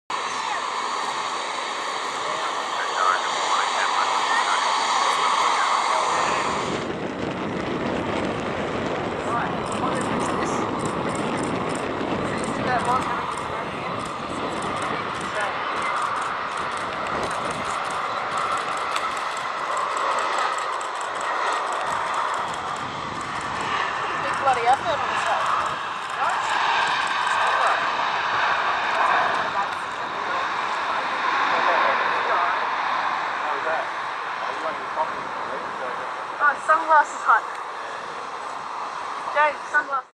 Jetstar (Sunglass Hut) Airbus A320-232 [VH-VFV] | Landing | Brisbane Airport
Welcome back to another Brisbane Airport Plane Spotting video!

Featured in this video is a Jetstar Airbus A320-232 in the SUNGLASS HUT livery, an advertisement for that store. Jetstar has quite a few planes with decals promoting companies, movies etc. at the moment making it just that little more interesting when going out and spotting to try and catch one.

Please watch in FULL 1080P HD!

||| AIRCRAFT INFO |||
Airline - Jetstar (JQ/JST)
Aircraft - Airbus A320-232
Registration - VH-VFV (Built 2013)
Engines - 2x International Aero Engines IAE V2527-A5 engines
Livery - SUNGLASS HUT decals
Flight # - Unknown
Route - Unknown
Runway - 01
Date - 21/02/16